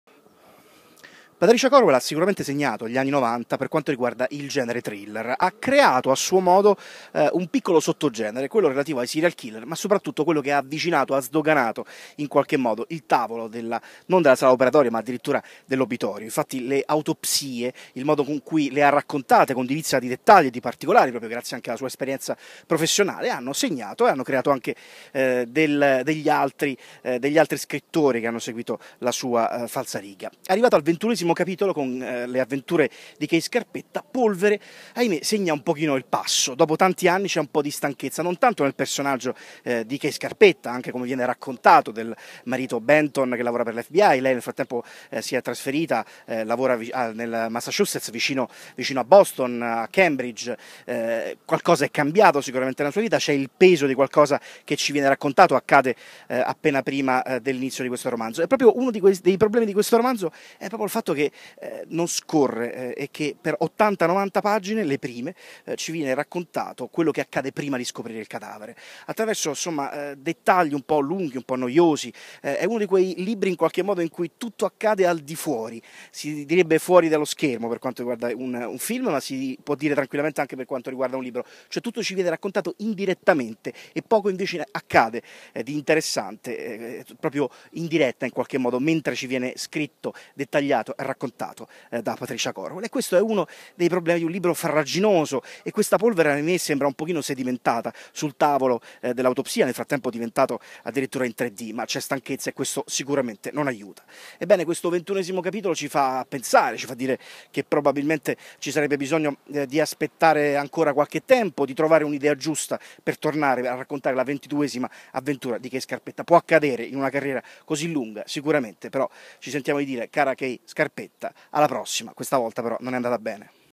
La video recensione del thriller Polvere di Patricia Cornwell
La video recensione del nuovo romanzo con protagonista Kay Scarpetta